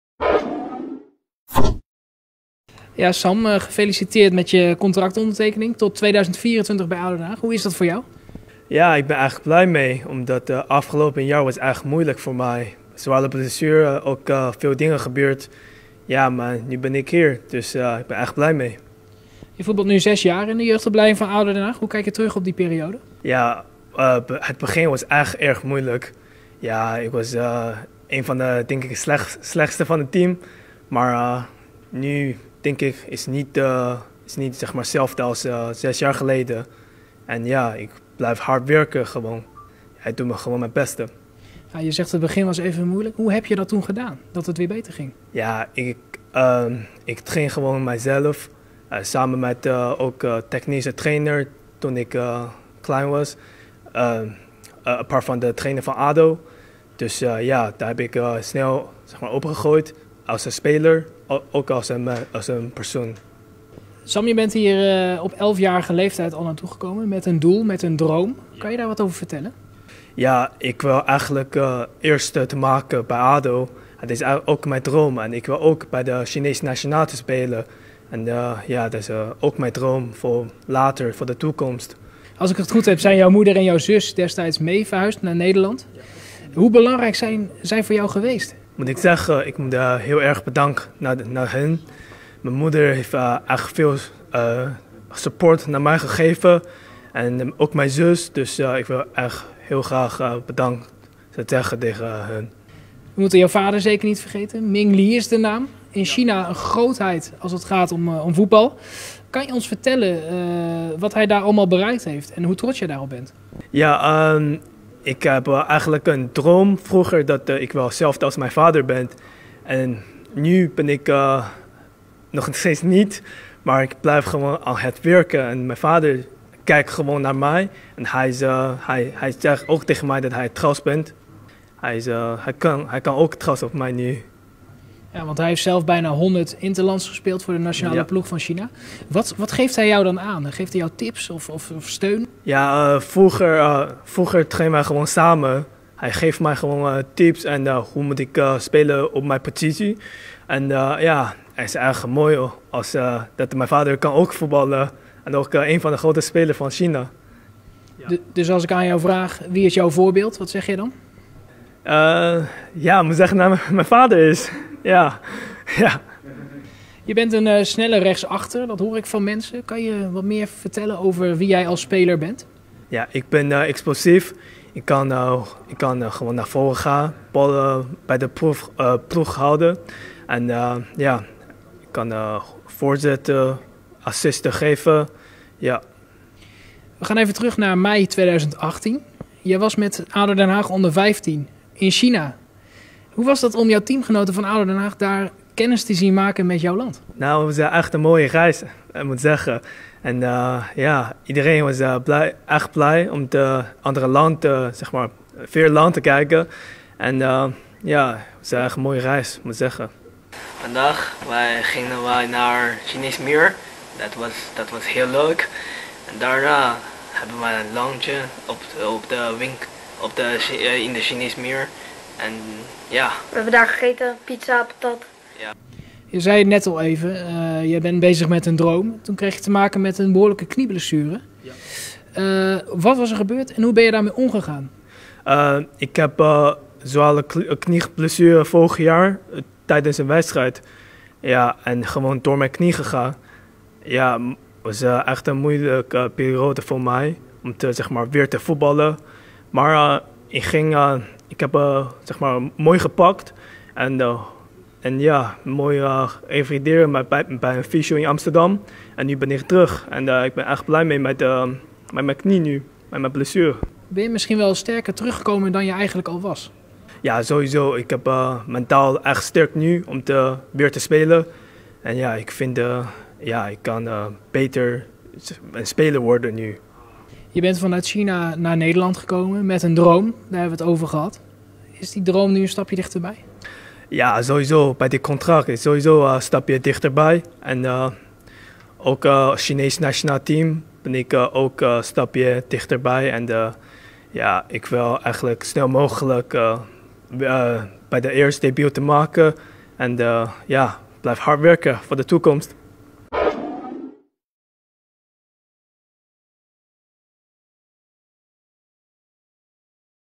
李嗣镕专访：我的偶像是爸爸 一直梦想为中国国家队出战
视频来源见水印。